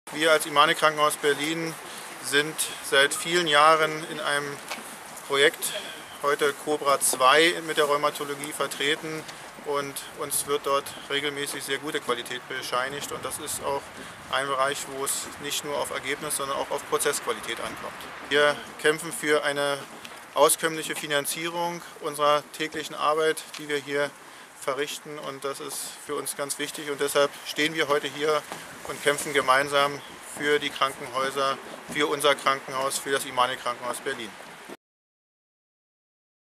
Immanuel Krankenhaus Berlin: Krankenhaus-Reform? So nicht!
Das Immanuel Krankenhaus Berlin unterstützt den Aktionstag der Deutschen Krankenhausgesellschaft "Krankenhaus-Reform- So nicht!" am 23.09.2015